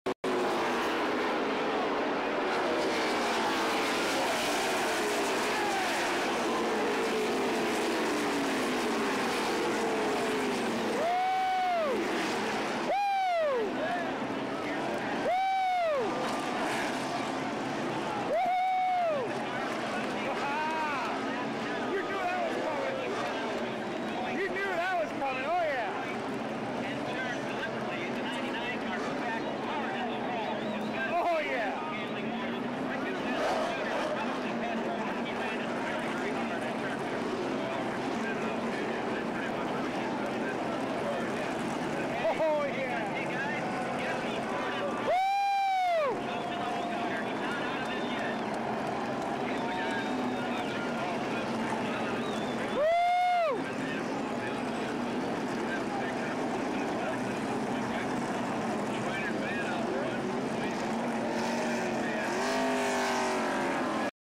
2008 ARCA @ Toledo - Speed and Stenhouse wreck hard
Point leader Scott Speed was put into the turn three wall by second-place point man Ricky Stenhouse and damaged the right side of his car. With the championship lost, Speed decided to make sure the guy who ruined his title hopes wasn't going to take home the trophy either. The result is a hard crash in turn one.